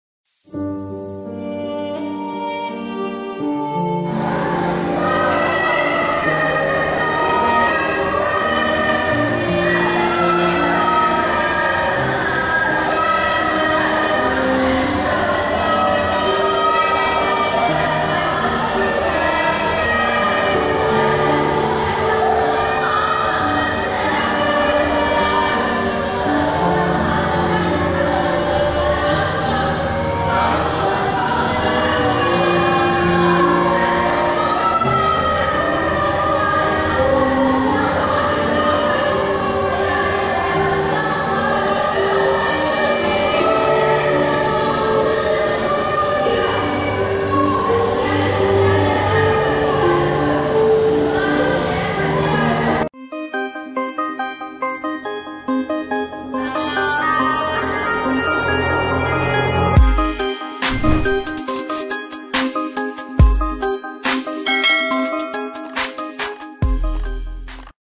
எப்பத்தா சிற்றாலயம்|Deaf Children Praying|பேசும் தெய்வத்திடம் பேசும் பேச்சுத்திறன் குன்றிய மாணவர்கள்
பேசும் தெய்வத்திடம் பேசும் பேச்சுத்திறன் குன்றிய மாணவர்கள் | எப்பத்தா சிற்றாலயம்-
தன் கல்விக்காக தன் குடும்பத்தினருக்காக இறைவனிடம் பிராத்தனை செய்யும் செவித்திறன்குன்றிய மாணவர்கள்.

நீங்கள் இறைவனிடம் பேசுகிறீர்களா?